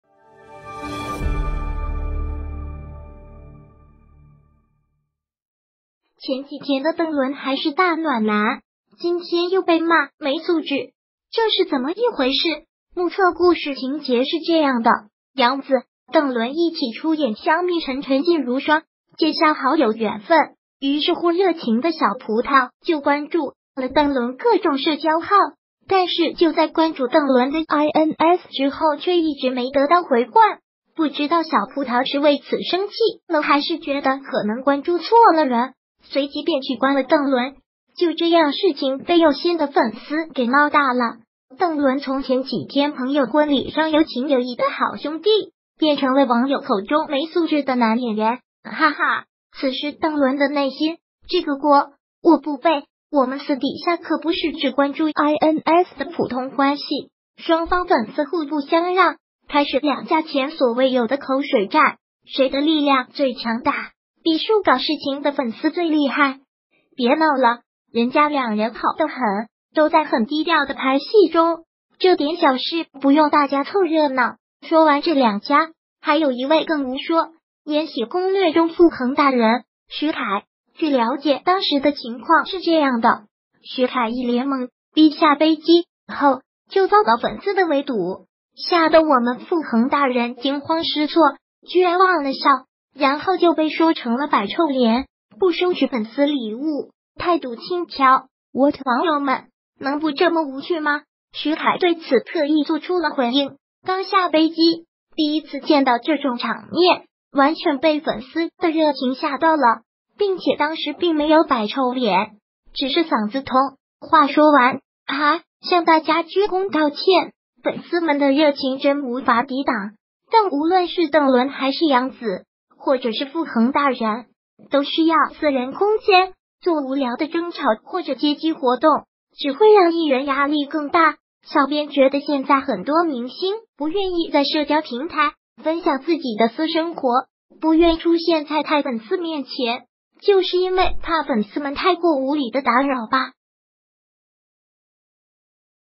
邓伦没回关杨紫ins，遭取关，遭骂：没素质！许凯回应摆臭脸！
前几天的邓伦还是大暖男，今天又被骂“没素质”，这是怎么一回事？ 目测故事情节是这样的，杨紫、邓伦一起出演《香蜜沉沉烬如霜》接下好友缘分。于是乎热情的小葡萄就关注了邓伦各种社交号，但是就在关注邓伦的ins之后却一直没得到回关。不知道小葡萄是为此生气了还是觉得可能关注错了人， 随即便取关了邓伦，就这样事情被有心的粉丝给闹大了，邓伦从前几天朋友婚礼上有情有义的好兄弟，变成了网友口中“没素质”的男演员。 哈哈，此时邓伦的内心：“这个锅，我不背！我们私底下可不是只关注ins的普通关系”,双方粉丝互不相让，开始两家前所未有的口水战！ 谁的力量最强大？必数搞事情的粉丝最厉害！ 别闹了，人家两人好的很， 都在很低调的拍戏中，这点小事不用大家凑热闹。 说完这两家，还有一位更无辜，《延禧攻略》中傅恒大人—— 许凯。 据了解当时的情况是这样的，许凯一脸懵逼下飞机后就遭到粉丝的围堵，吓的我们傅恒大人惊慌失措，居然忘了笑？？？然后就被说成了摆臭脸？不收取粉丝礼物，态度轻佻，What？ 网友们，能不这么无趣吗？许凯对此特意做出了回应：“刚下飞机，第一次见到这种场面，完全被粉丝的热情吓到了，并且当时并没有摆臭脸，只